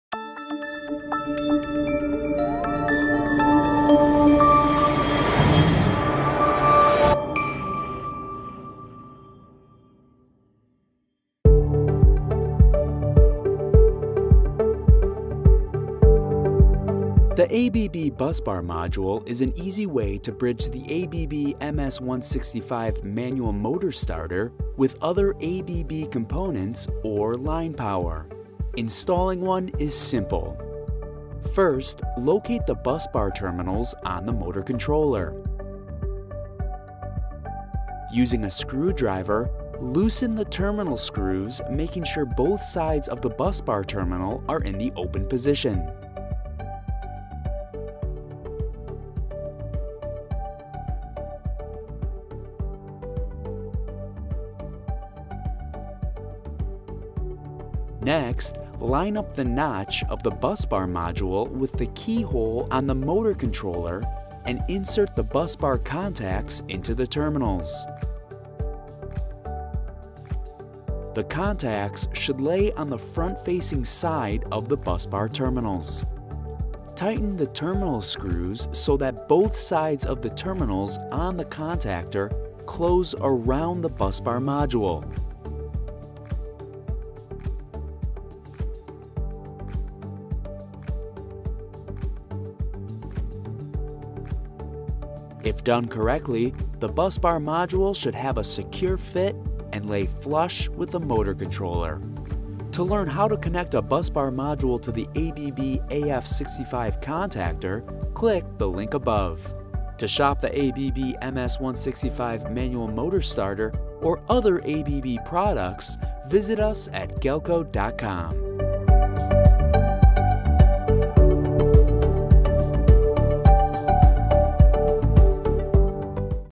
How to Connect a Busbar Module to an ABB MS165 Manual Motor Starter | Galco
The ABB busbar module is an easy way to bridge the ABB MS165 manual motor starter with other ABB components or line power. In this video, we will show you how simple it is to install one.